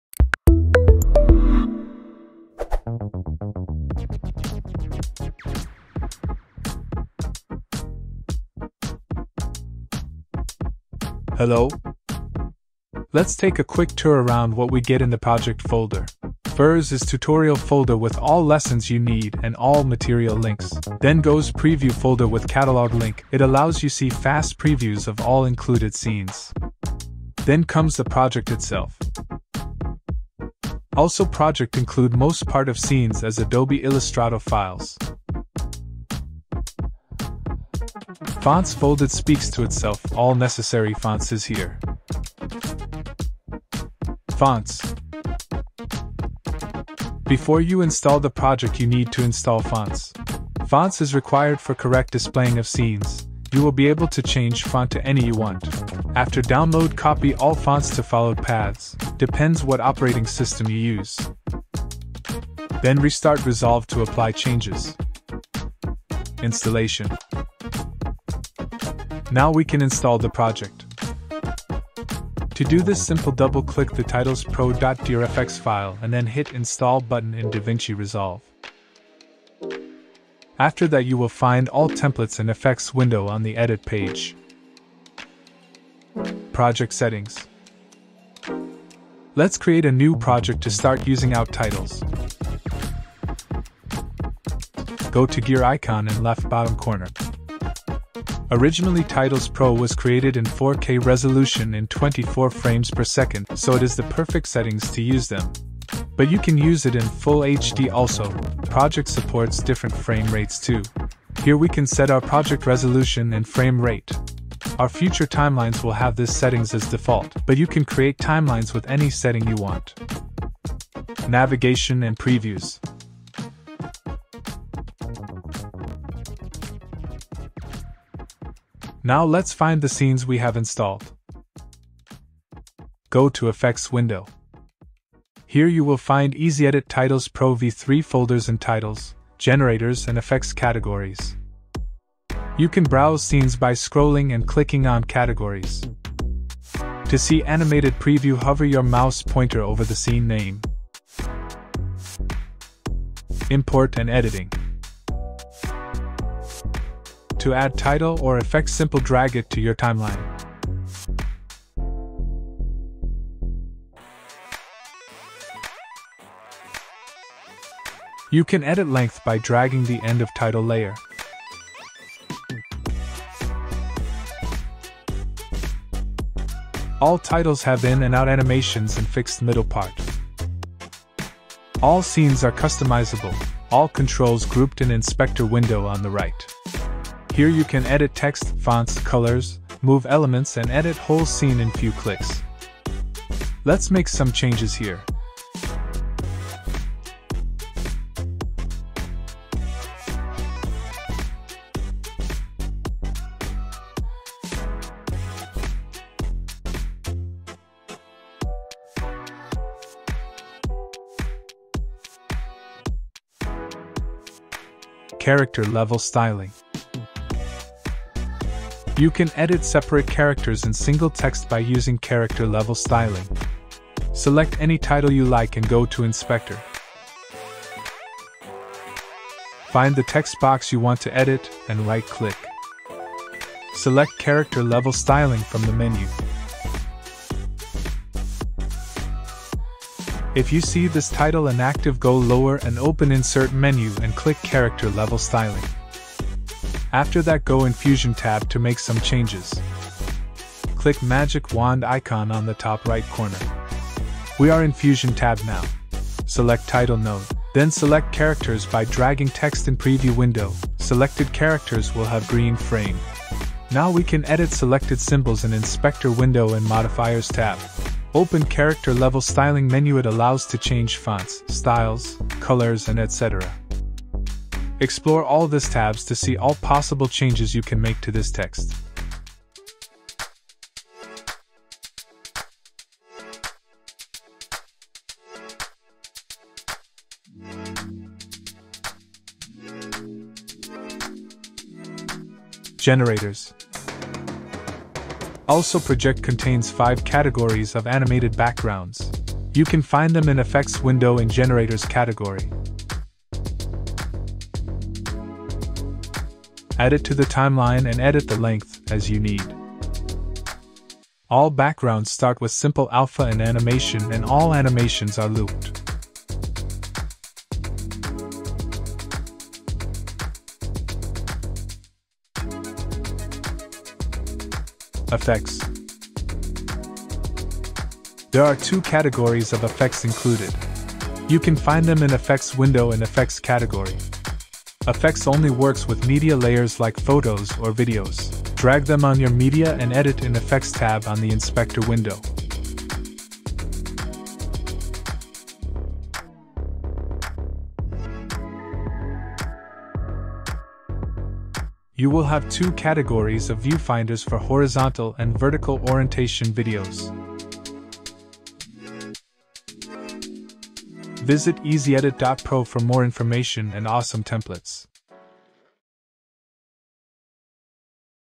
Titles Pro V3 | Get Pro Edits in DaVinci Resolve in MINUTES
Create animated title and animate typography with Titles Pro! Liquid text, kinetic text, animated backgrounds, lower thirds, viewfinders, countdowns and many more DaVinci Resolve templates!

Titles Pro is updated for Version 3!🎉
Here you can find more than 1500 animated templates, such as:

 1️⃣ Minimal Titles, Lower Thirds, Sale, Quotes, Callouts - available in the project since the V1; will greately emphasize your project with unique design. Perfectly suits for vlogs or ads - your video will stand out everywhere!

2️⃣ Liquid, Digital, Glitch, Kinetic Titles and Ready-Scenes —  were added in the V2 update; now you can step to the next level with professional animation - use trendy titles for your digital artwork, social media posts, branding projects, freelance for clients or just for fun!

3️⃣ Color, Fashion, Sports, Cinematic, Countdowns, Stomps, Typographic Details, Camera Viewfinders, Backgrounds - are the new categories from our fresh update, V3! Promote your clothing brand or a lookbook with Fashion Category. Run a sport channel with Sports templates. Make a professional movie or trailer with Cinematic titles. Use customized meters, timers and clocks in our new Countdowns category. Create a dynamic title, animated by a character, word or line with the Stomps. Add spice to your design with the Typographic Details Category. Use new camera Viewfinders to add a filming effect. And finally the Backgrounds - different gradients and shapes, divided into light, dark and tinted. Now you can use these animated templates in any area of your work and create a masterpiece never seen before!

Another great feature of Titles Pro - you can combine templates together.
Mix & Match elements to get interesting combinations of templates and save time. You can mix any title with any background and any details template - we designed them to match and work for you.

The project is compatible with 4K, so your titles will look sharp even on a big screen!
Other convenient features are preview to all scenes, auto-resize of compositions and text editing in Essential Graphics panel. Our team has made dedicated work to make this update convenient for most users - from pro editors to beginners.

Use it anywhere: Youtube, Instagram Stories or Posts, Blogs, Presentations, Advertising, TV, Movies, Music Videos, Tik Tok or product promos! Titles Pro is designed to suit any task you can possibly have…see for yourself!

And even more than that - all future updates are FREE.

Timing:
00:00 - Intro
00:08 - Overview
00:43 - Fonts
01:07 - Installation
01:27 - Project Settings
02:00 - Navigation and Previews
02:34 - Import and Editing
03:36 - Character Level Styling
04:52 - Generators (Backgrounds)
05:25 - Effects